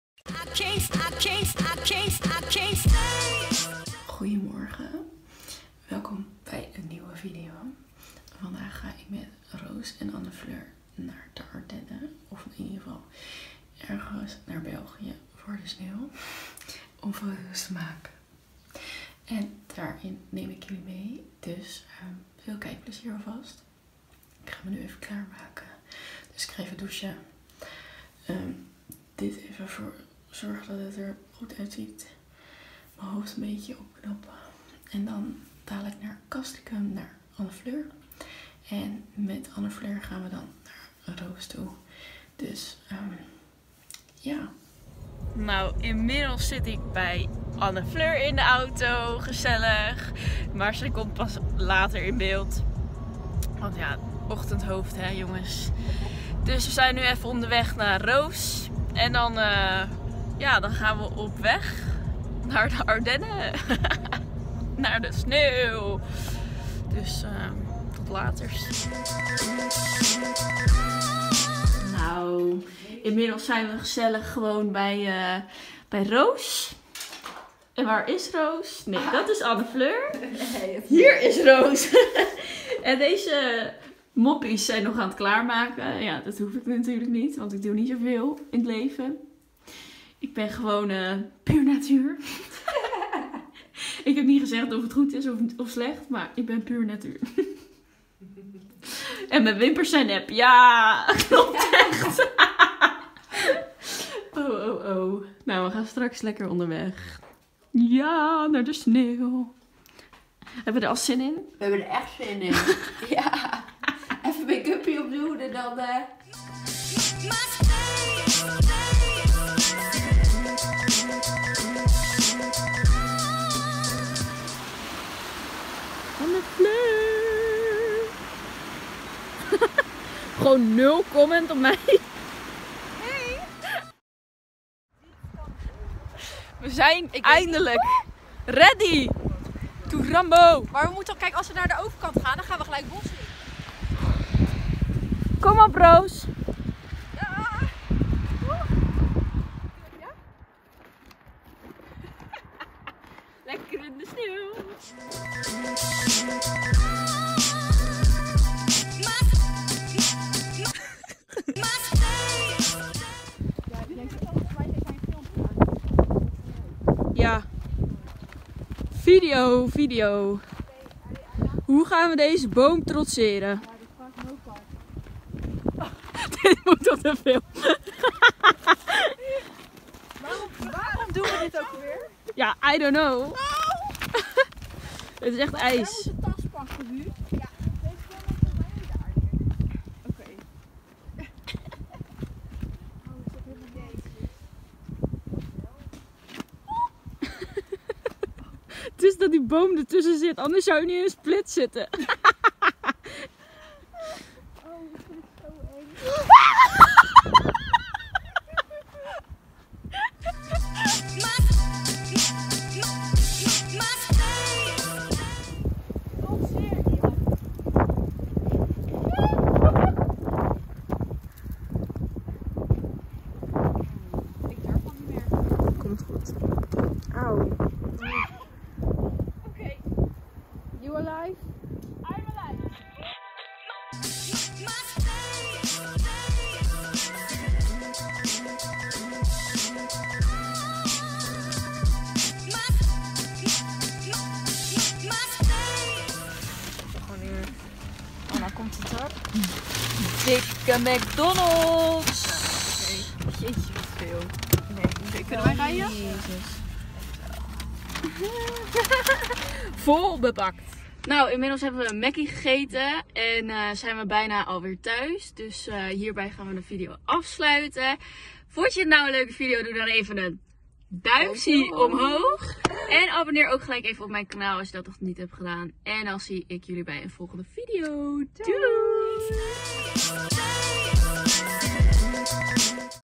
Snow trip | with the girls
Samen met Roos en Anne-Fleur zijn we naar België gereden om van de sneeuw te genieten en nieuwe content te schieten voor onze insta accounts.